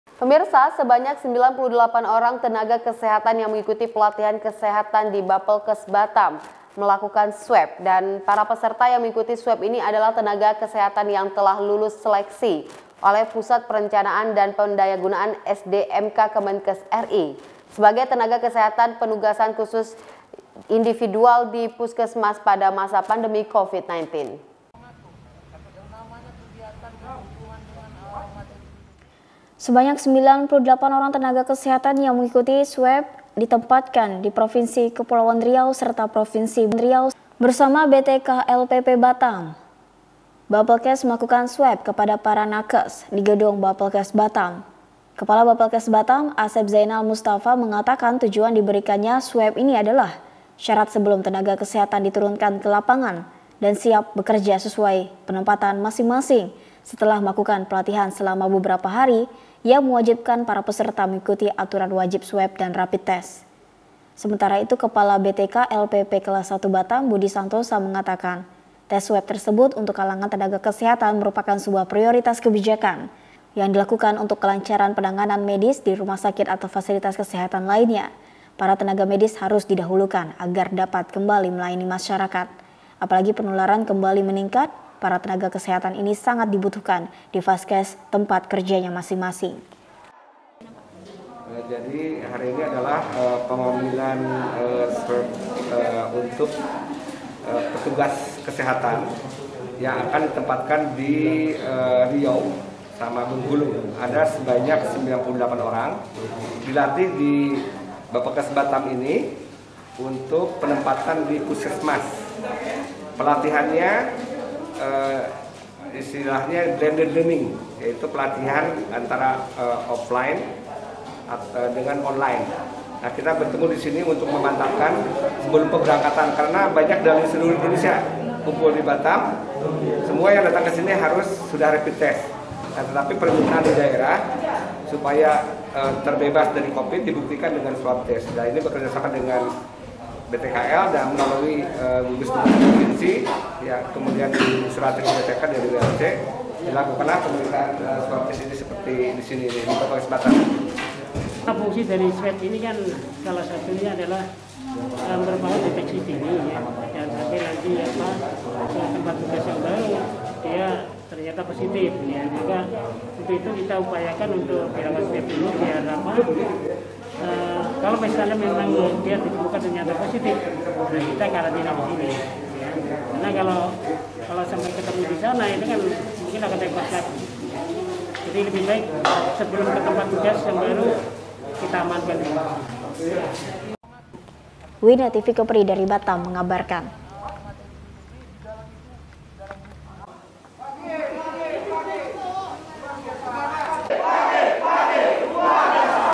USAI IKUTI PELATIHAN 98 TENAGA KESEHATAN LAKUKAN TEST SWAB DI BAPELKES - TV KEPRI
//Sebanyak 98 Orang Tenaga Kesehatan Yang Mengikuti SWAB ini akan Ditempatkan di Provinsi Riau Serta Provinsi Bengkulu.

//Bersama BTKLPP Batam Bapelkes Melakukan SWAB Para Nakes ini di Gedung Bapelkes Batam //Kepala BAPELKES Batam  Asep Zaenal Mustafa Mengatakan Tujuan Diberikan Ya SWAB adalah Syarat Sebelum Tenaga Kesehatan ini Diturunkan Ke Lapangan dan Siap Bekerja Sesuai Penempatan Masing2 Setelah Melakukan Peltihan Selama Beberapa Hari Yang Mewahibkan Para Peserta Mengikuti Aturan Wajib SWAB dan Rapid Test // Sementara Itu Kepala BTKLPP Kelas I Batam Budi Santosa Mengatakan Test Swab Tersebut Untuk Kalangan Tenaga Kesehatan Merupakan Sebuah Prioritas Kebijakan ini Dilakukan  Untuk Kelancaran Penanganan Medis di Rumah Sakit atau Fasiltas Kesehatan Lainnya  Para Tenaga Medis Harus Didahulukan agar Dapat kembali Melayani Masyarakat. Apalagi, Penularan Kembali Meningkat  Para Tenaga Kesehatan  ini Sangat Dibutuhkan di Faskes Tempat Kerjanya Masing-masing.